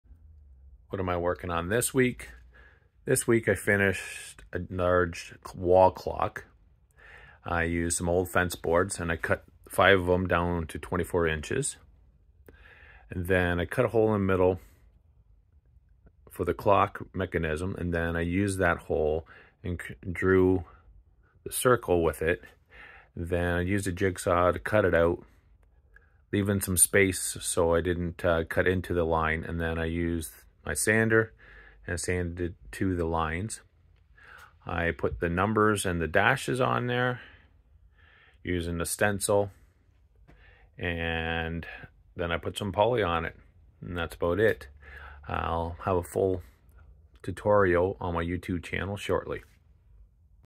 What am I working on This week!!! Episode 11
What am I working on This week!!! Episode 10 - I am working on making a large Farmhouse wall clock!!

Dewalt Sander

Black Stain

Special Walnut Stain

Foam paint brushes

Dewalt Drill

Picture Frame Hangers

Shelf Brackets

Scroll saw